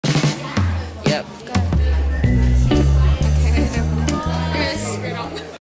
Burning Steelwool... Ish... Zoomed out
The same video as the previous Steelwool burning except its zoomed out